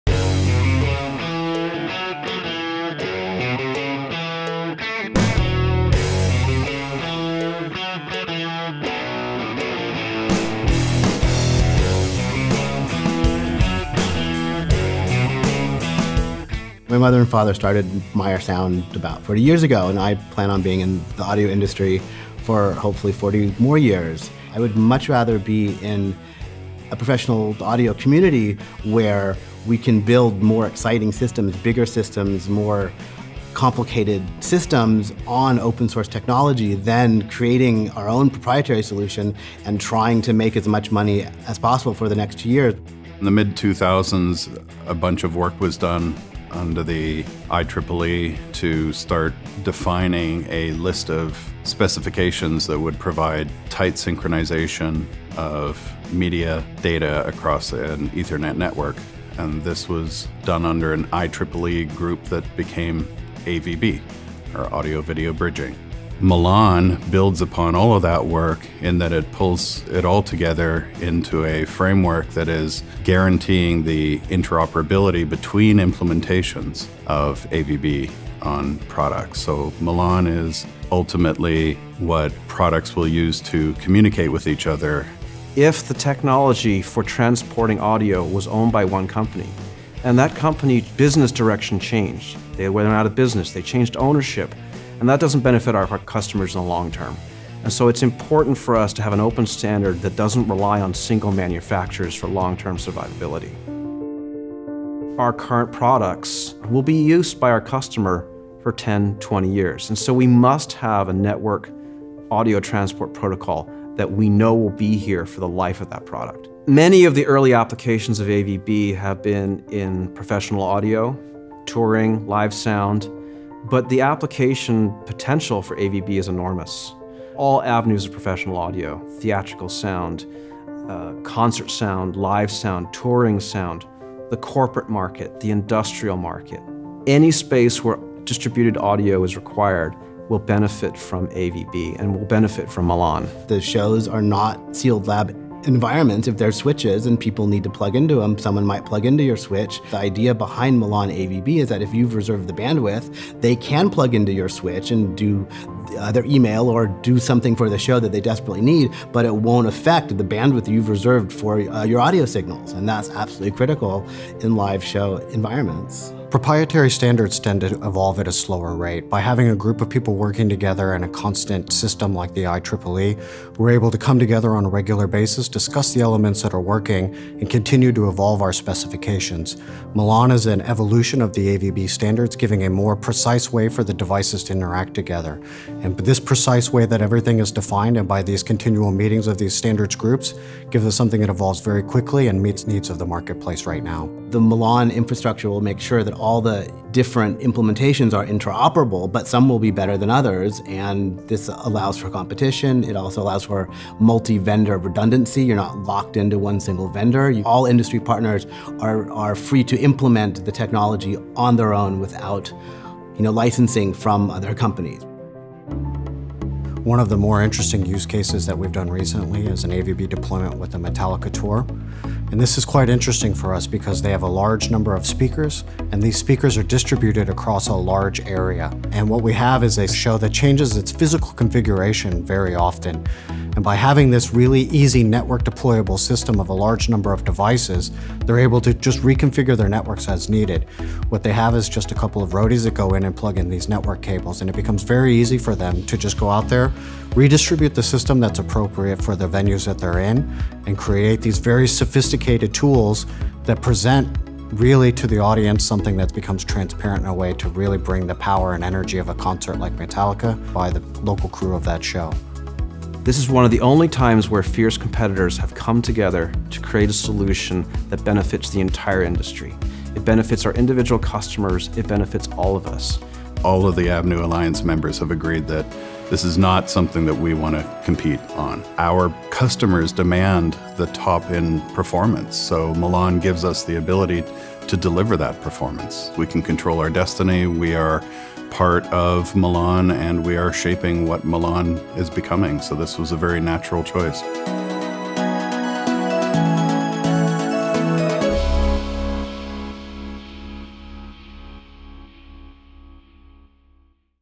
Meyer Sound - Why Milan
Avnu Alliance and Milan workgroup member Meyer Sound discusses the benefits of implementing and supporting the growing Milan protocol for pro AV.